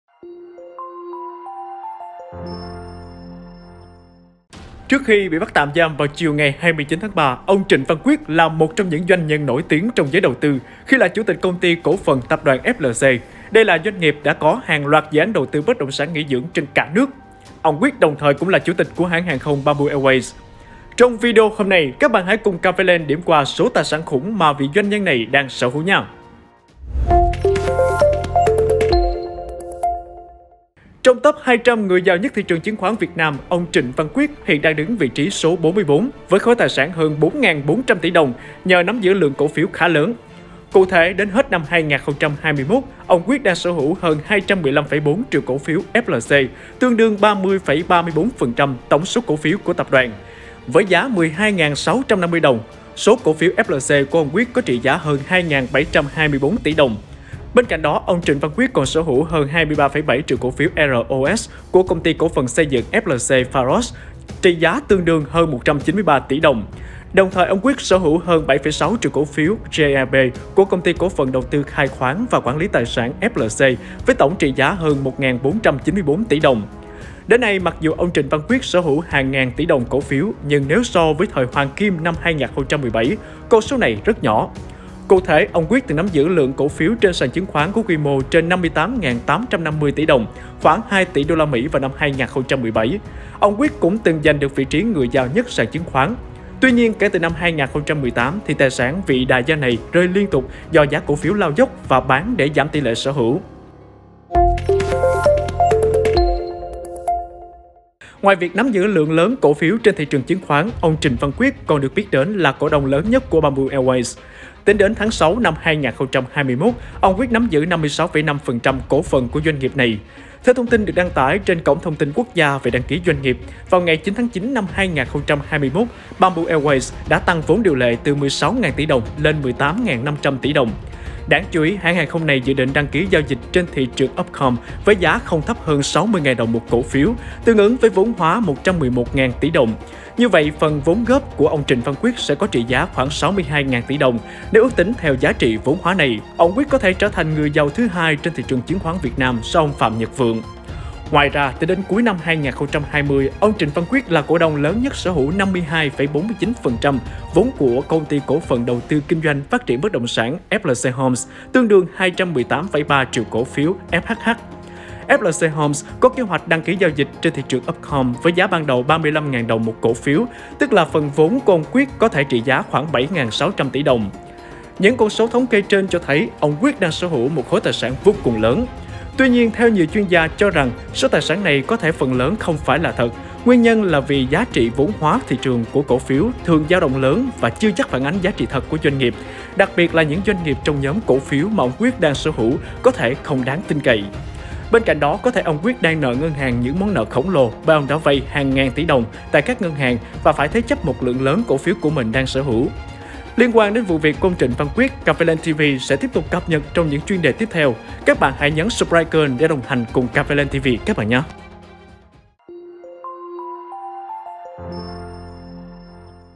Khối tài sản của ông Trịnh Văn Quyết, Chủ tịch FLC như thế nào? | CAFELAND
Trước khi bị bắt tạm giam vào chiều ngày 29/3, ông Trịnh Văn Quyết là một trong những doanh nhân nổi tiếng trong giới đầu tư khi là Chủ tịch Công ty cổ phần Tập đoàn FLC, đây là doanh nghiệp đang có hàng loạt dự án đầu tư bất động sản nghỉ dưỡng trên cả nước. Ông Quyết đồng thời cũng là Chủ tịch của hãng hàng không Bamboo Airways.

Trong video hôm nay, các bạn hãy cùng Cafeland điểm qua số tài sản “khủng” mà vị doanh nhân này đang sở hữu nhé.

Xem thêm: Soi dự án Unico Thăng Long nằm dọc quốc lộ 13, có giá bán 22 triệu đồng/m2